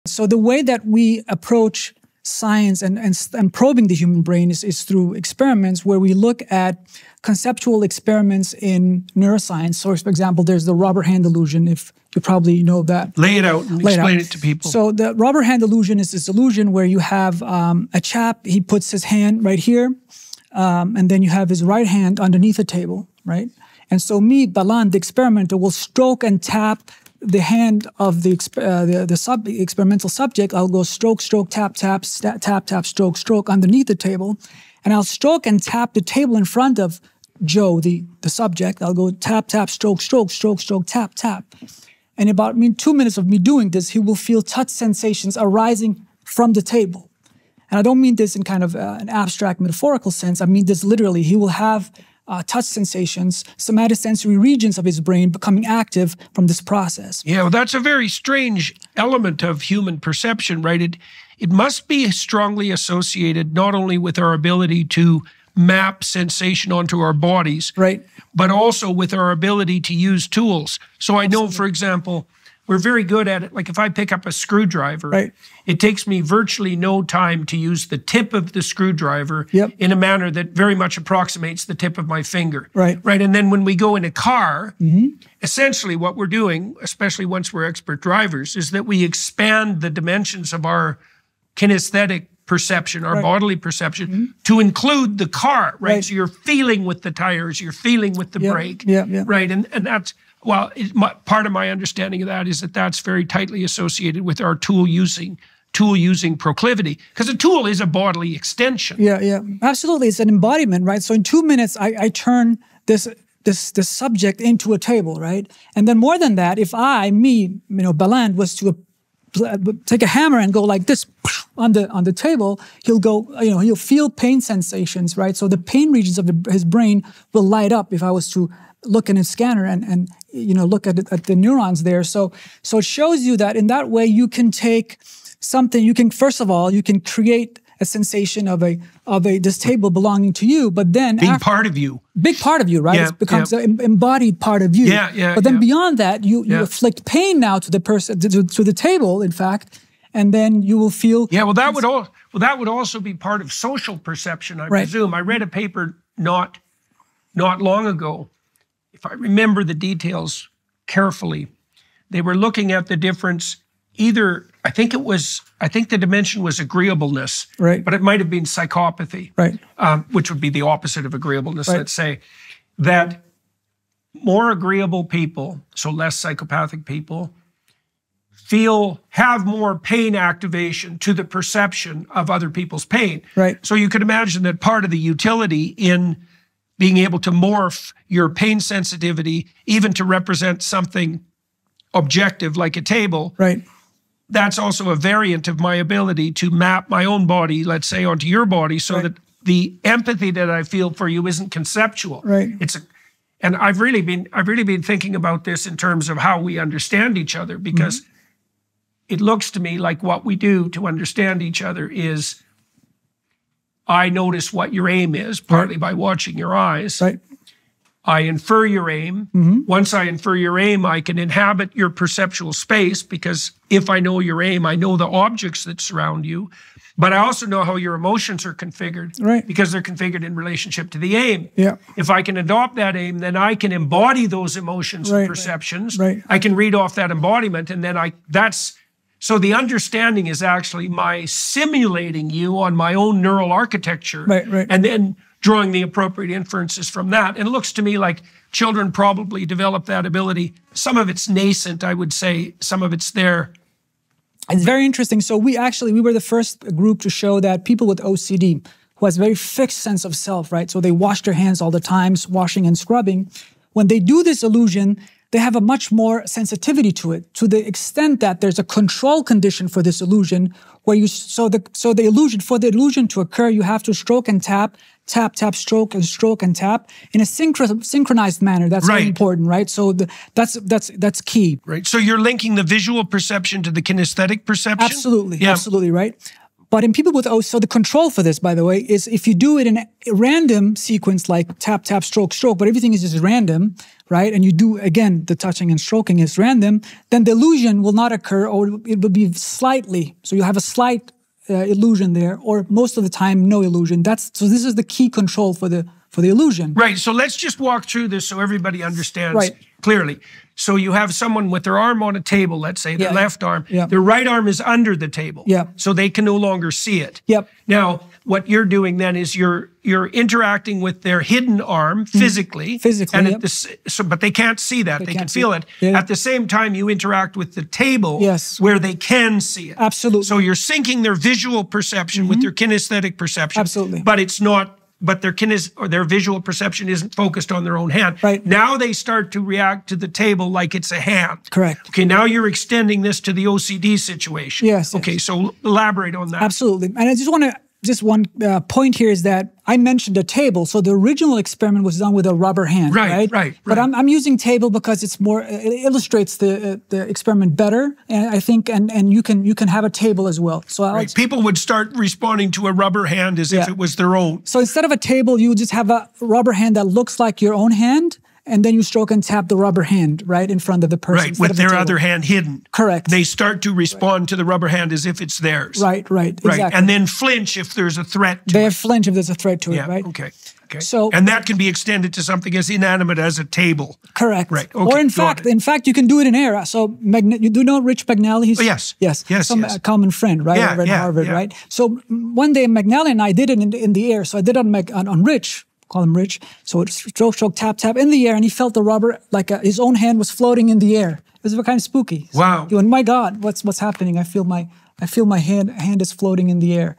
The Experiment That Will Trick Your Mind - Jordan Peterson
Jordan Peterson talks with Baland Jalal about how does the rubber hand experiment explains the high plasticity of the human brain.